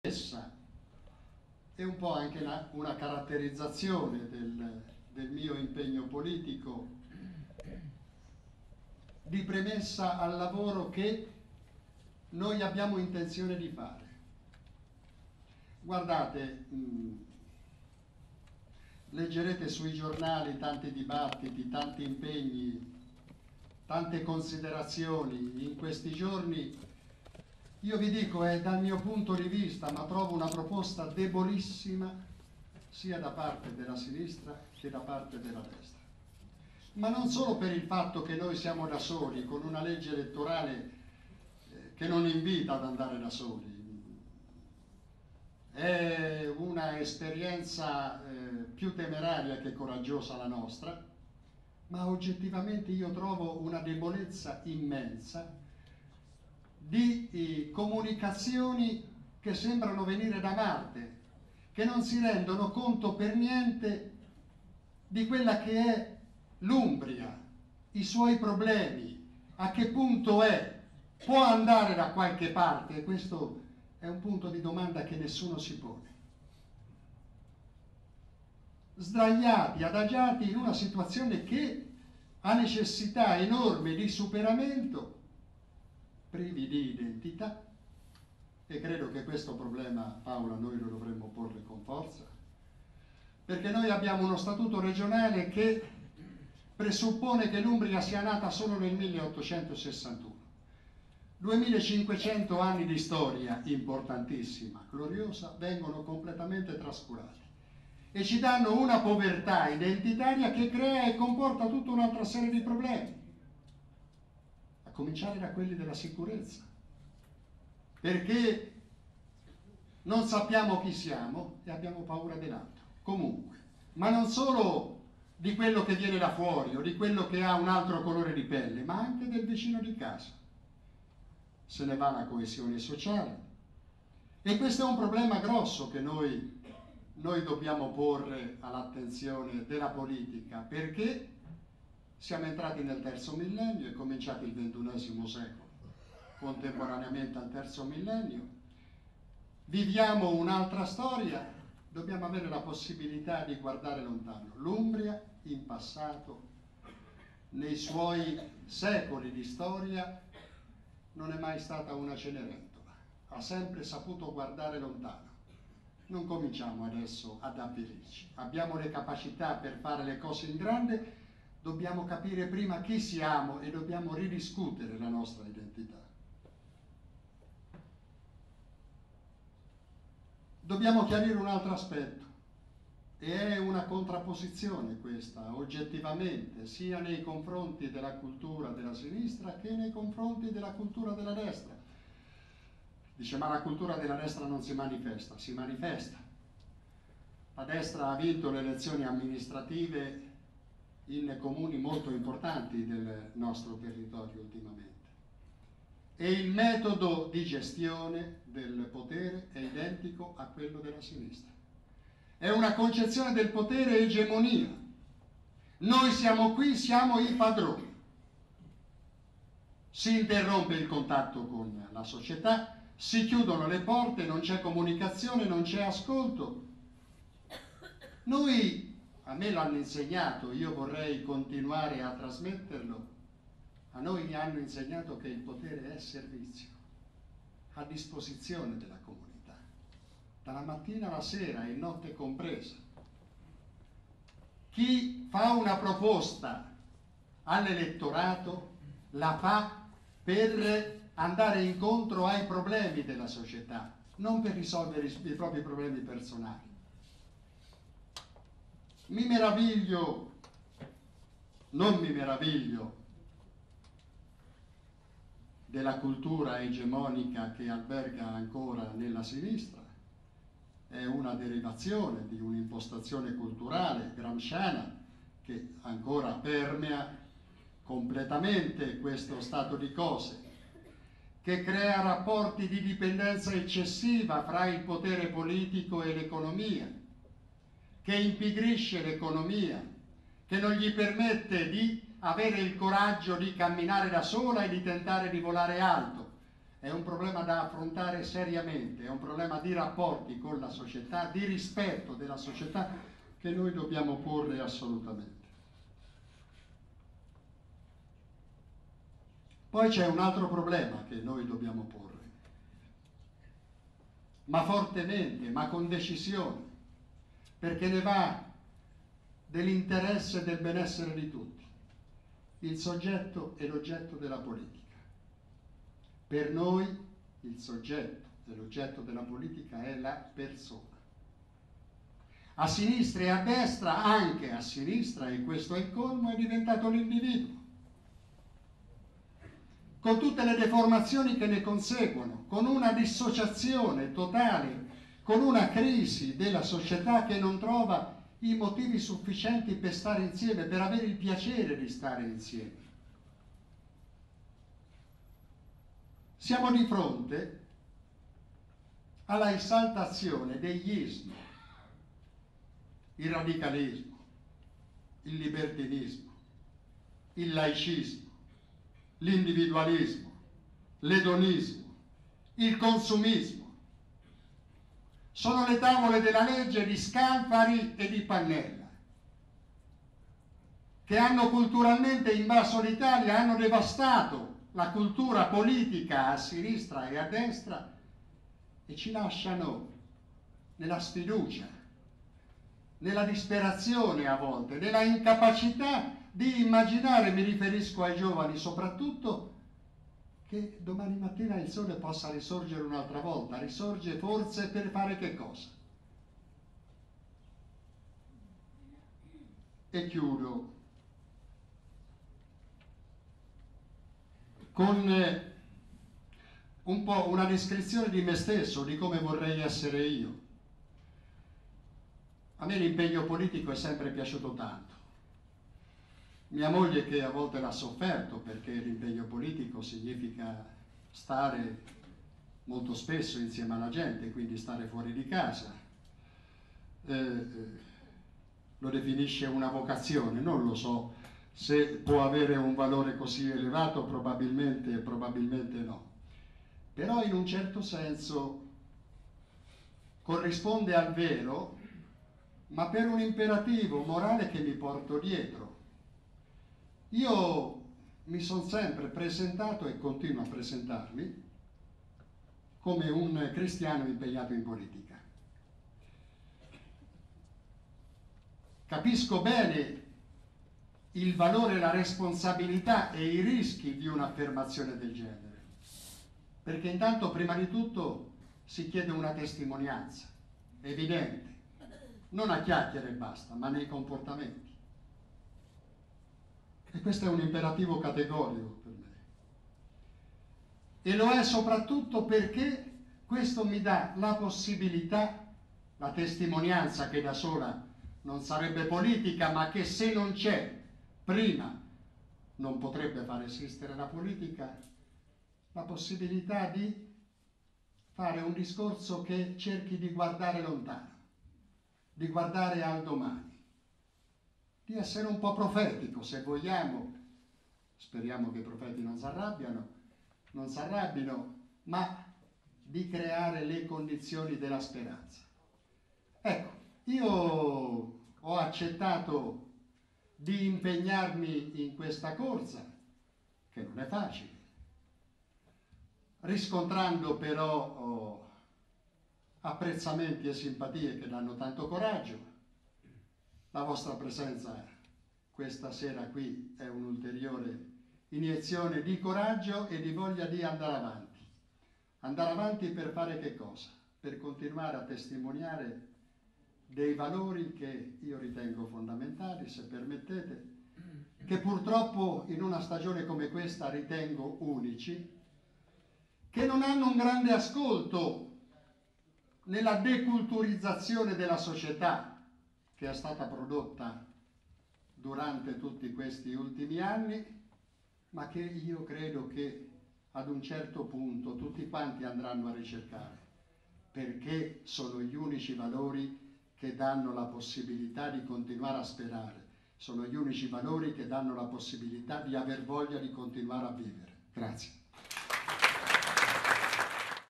Giulio Cozzari presenta il programma UDC per le prossime elezioni regionali
Giulio Cozzari interviene al Centro Mater Gratiae di Montemorcino (Perugia) per presentare il programma  elettorale UDC per le prossime elezioni regionali. All'incontro ha partecipato la candidata Presidente Paola Binetti.